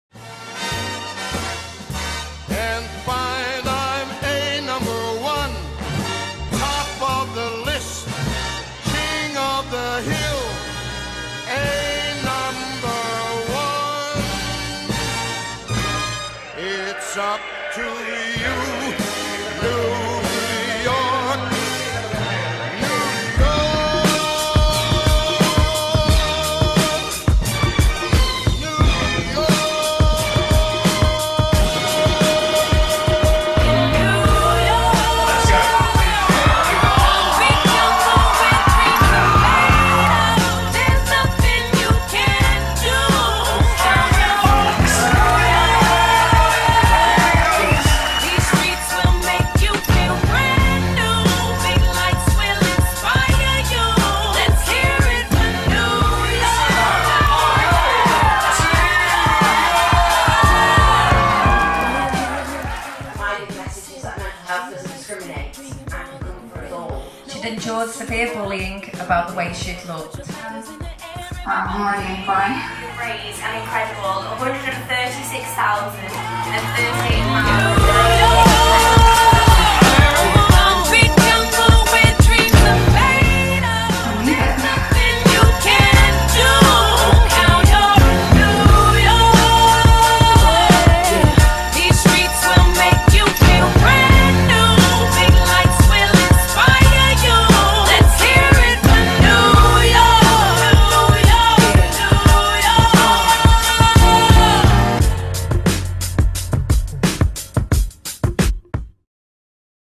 FFM Winter Waltz 2021
Our inaugural Winter Waltz Gala Dinner took place on Friday 26th November 2021 at The Mere Golf Resort and Spa and really was a 'fairytale' of a night as we raised over £136,000 for the St. James's Place Charitable Foundation!